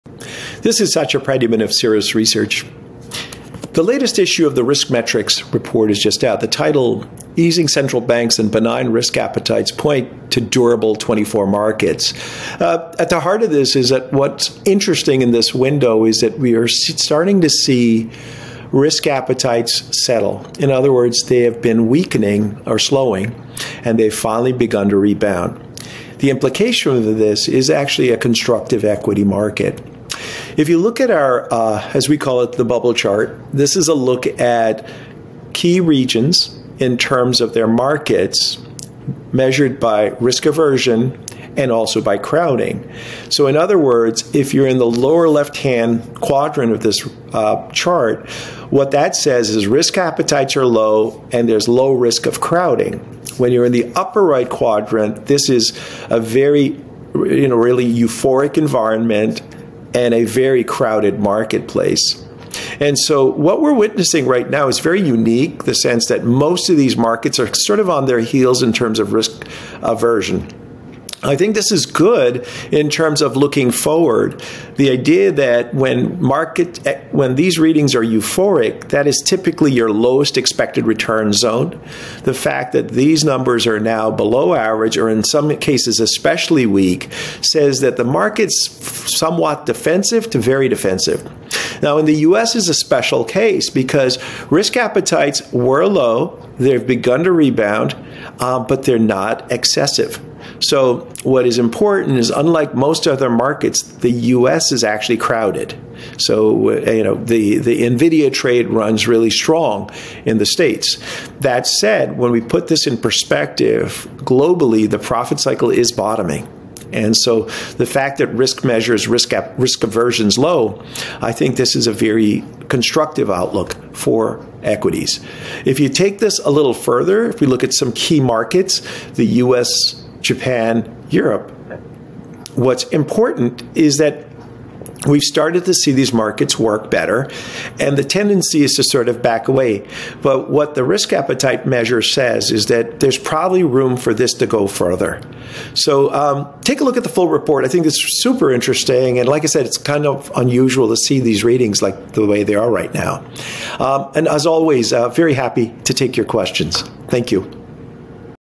Cirrus Risk Metrics: Easing Central Banks and Benign Risk Appetites Point to Durable 2024 Markets
Global risk appetites have faded to more constructive levels, adding to our positive view for equities. In just three months, risk appetites have reached lows not seen in almost a year, the most extreme in LatAm, Canada and Japan. When risk aversion becomes extreme, one can expect strong equity market returns in the year to follow. The positive risk backdrop, combined with bottoming profits and easing central banks, bodes well for global equities. In fact, the recent rate cut by the Swiss Central Bank has heightened expectations for other rate cuts in the world to follow. Across the globe, we remain cautious of momentum shares in the US due to crowding concerns and equities in China as fundamentals remain unattractive. Japan remains an attractive market in the global landscape.